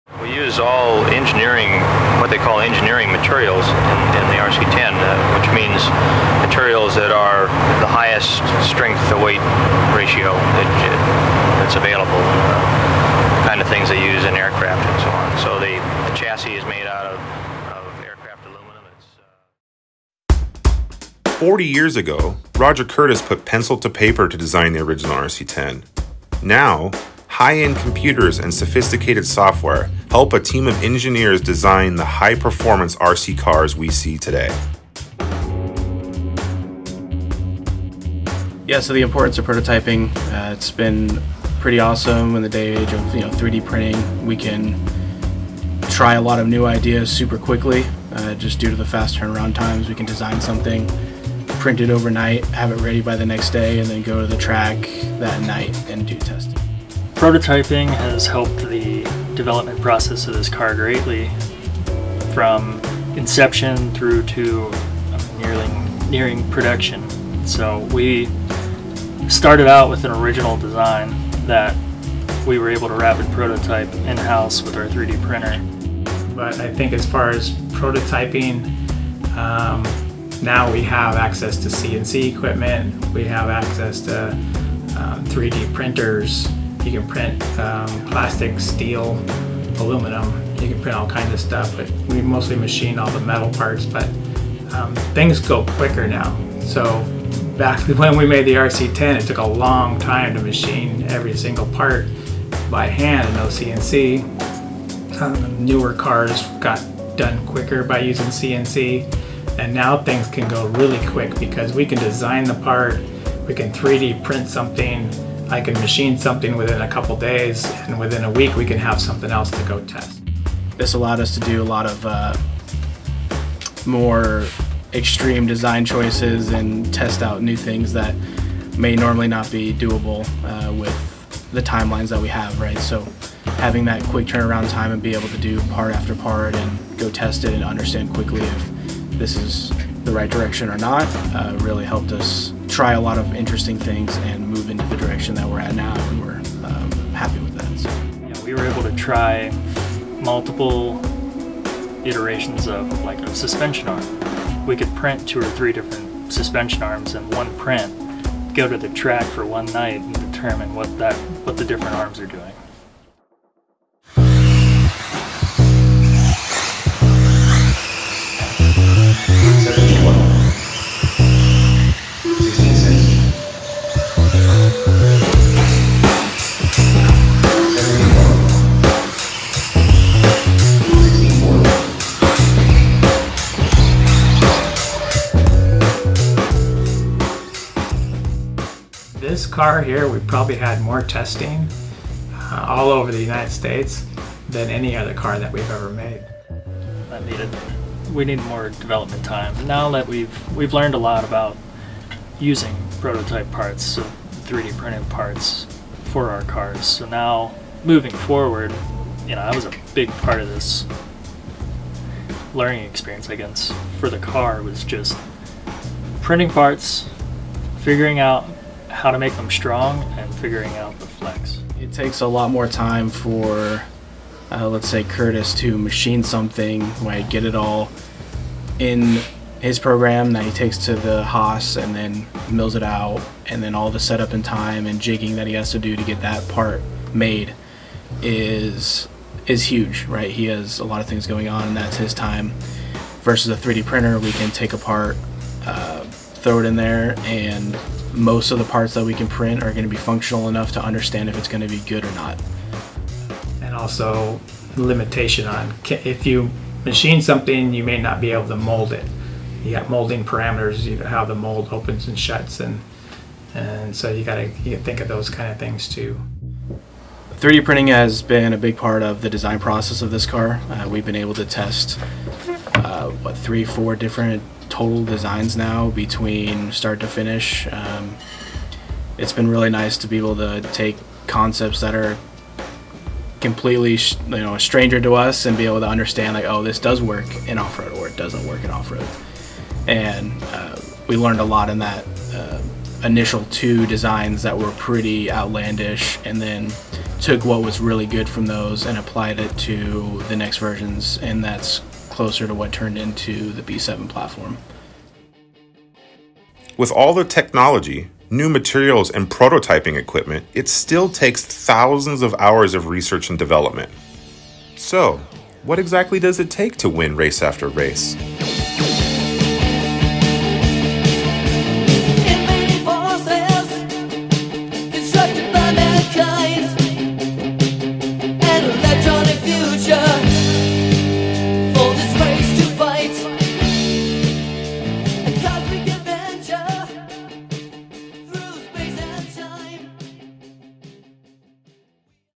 PART 3 of 7 - RC10B7 Video Series: Design Technology
In part three of our RC10B7 video series, we talk about the design and technology used during the B7 development.